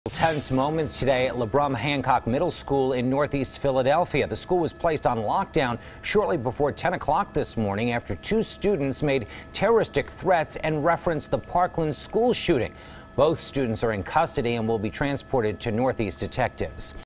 Labrum Hancock Middle School Placed On Lockdown After 2 Students Allegedly Made Threats
Both students are in custody and will be transported to Northeast detectives.